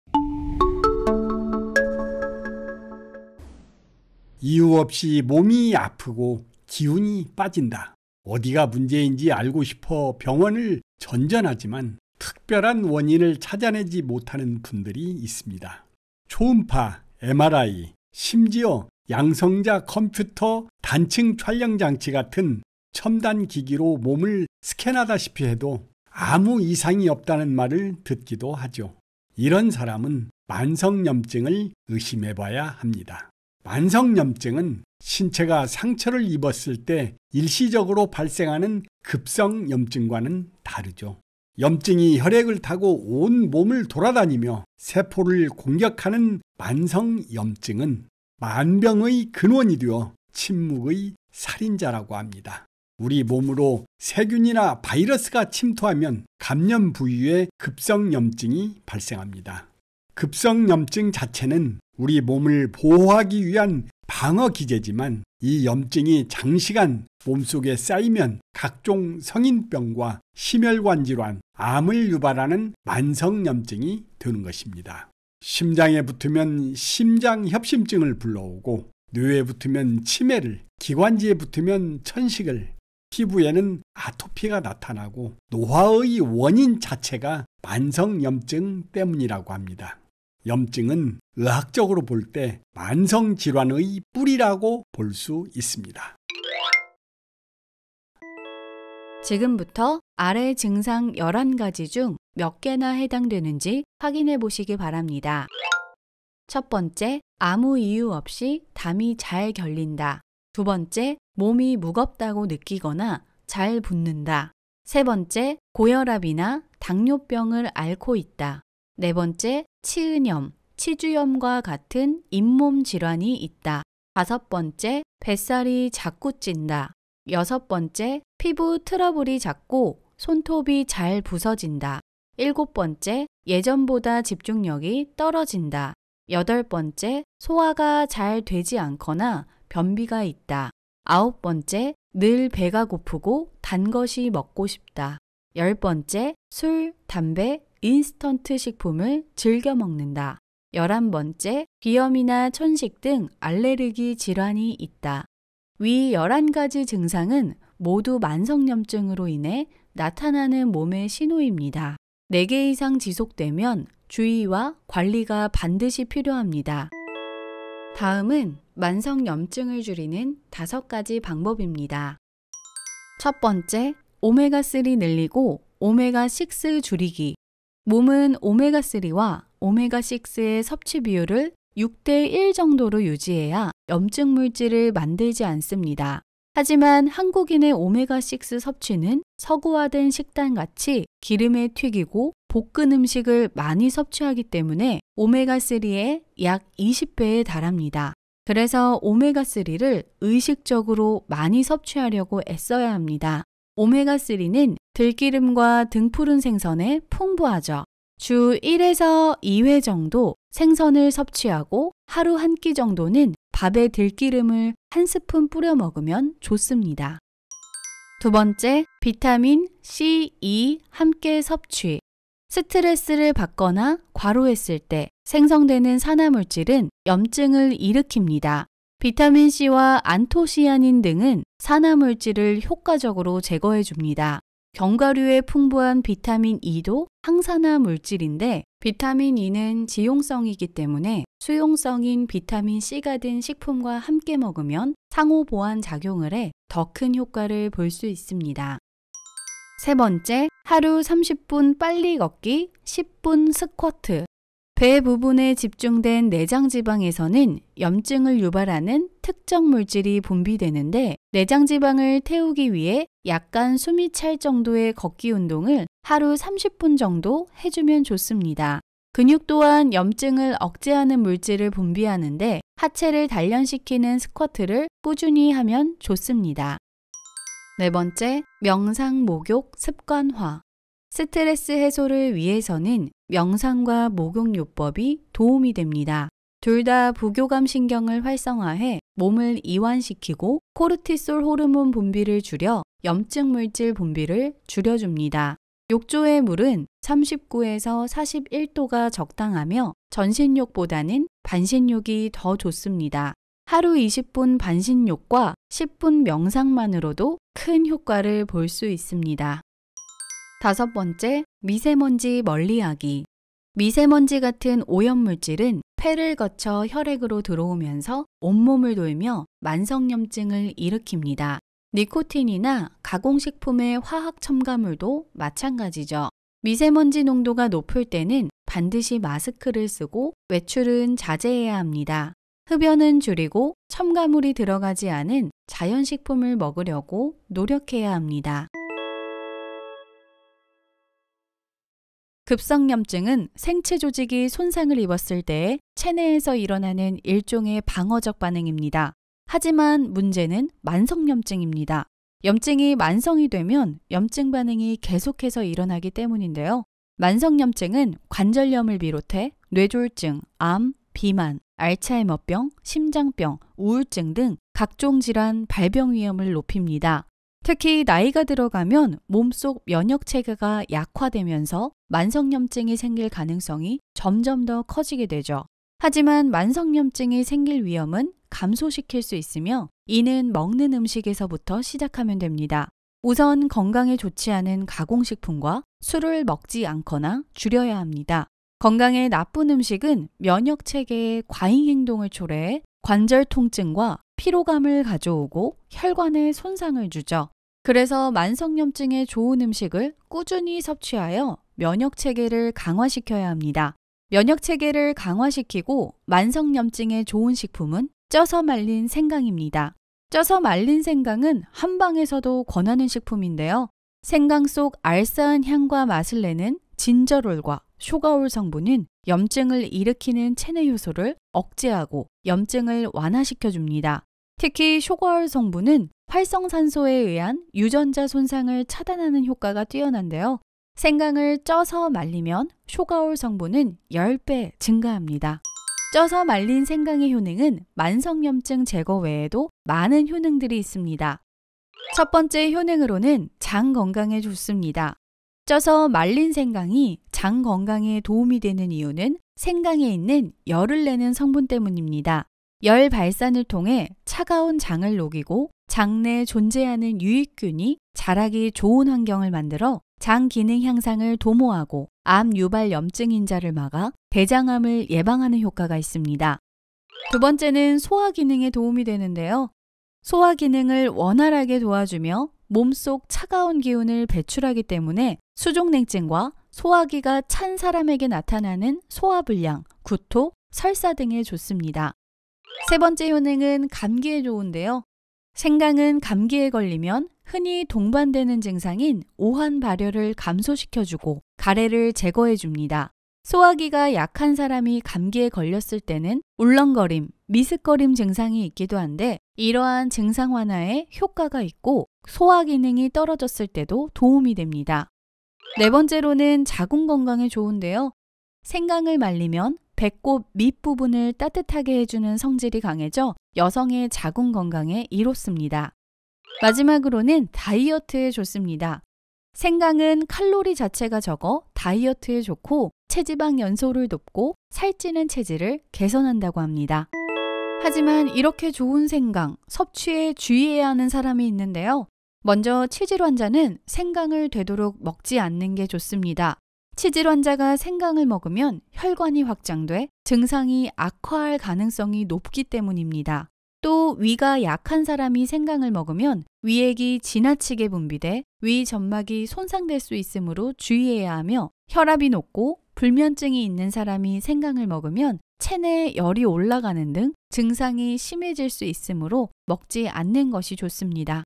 만성염증 초기증상 11가지중 4가지 이상 지속되면 병원으로 달려가세요
이유 없이 몸이 아프고 기운이 빠진다
어디가 문제인지 알고 싶어 병원을 전전하지만
특별한 원인을 찾아내지 못하는 분들이 있습니다
초음파, MRI(자기공명영상촬영장치)
심지어 양성자 컴퓨터단층촬영장치(PET-CT) 같은
첨단 기기로 몸을 스캔하다시피 해도
아무 이상이 없다는 말을 듣기도 하죠
이런 사람은 만성 염증을 의심해 봐야 합니다